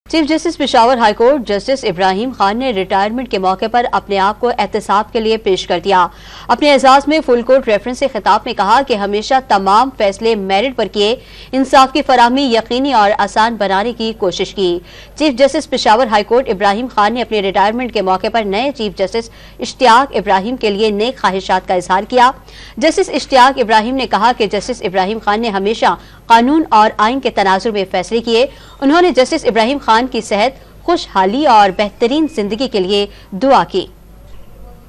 Chief Justice Peshawar High Court Justice Ibrahim Presented Himself For Accountability | Dawn News
Chief Justice Peshawar High Court Justice Ibrahim Khan Presented Himself For Accountability | Dawn News

چیف جسٹس نے خود کو احتساب کیلئے پیش کردیا